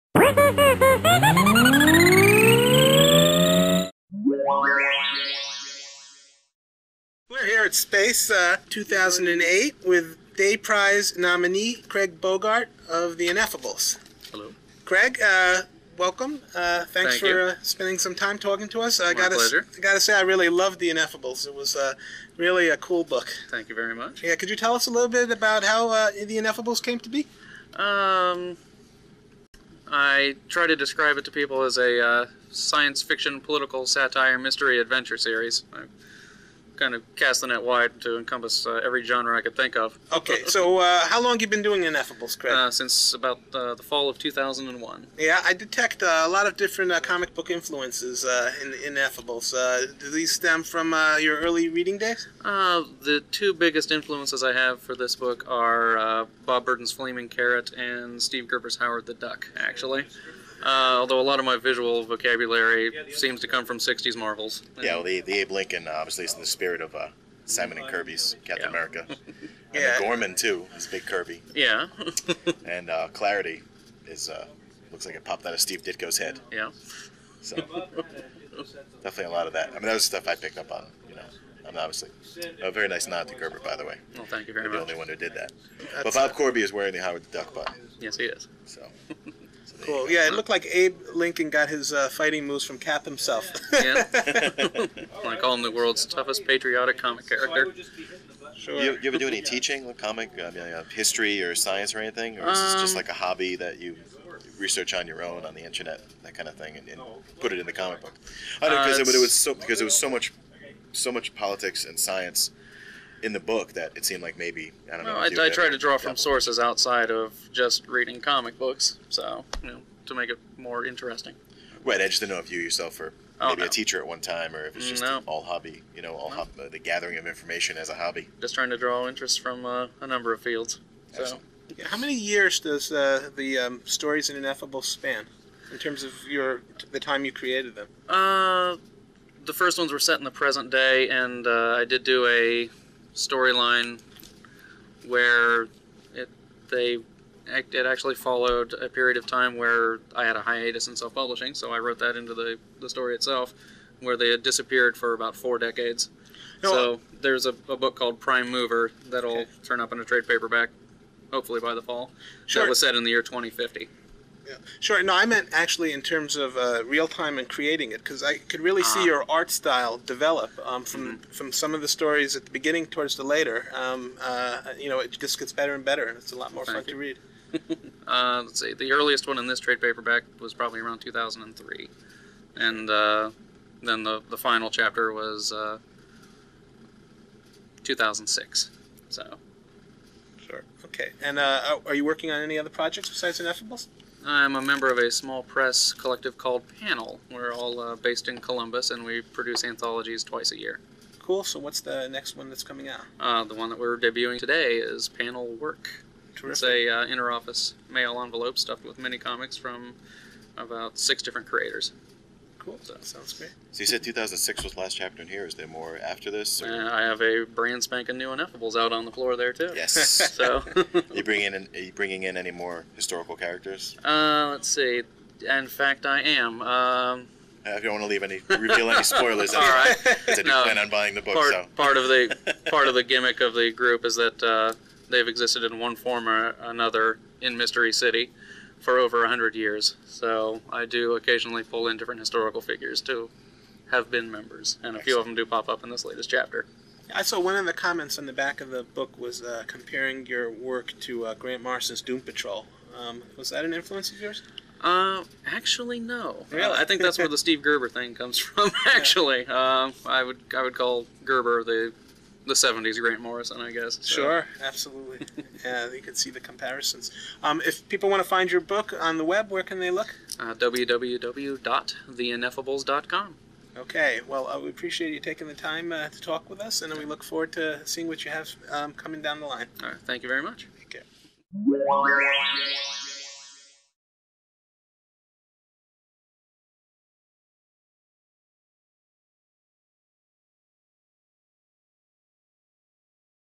SPACE 2008 Interview: Craig Bogart (The Ineffables)
SPACE 2009 (The Small Press and Alternative Comics Expo) is almost upon us, and in anticipation we offer up a series of interviews with regular attendee comic creators, most of whom were Day Prize (now the Space Prize) nominees.

For more information on SPACE 2009 being held at the Aladdin Shrine Center in Columbus, OH on April 18 & 19, 2009,

This is an interview with Graig Bogart, creator of The Ineffables!

Hosted by Lenny Cooper and Jeff Tundis.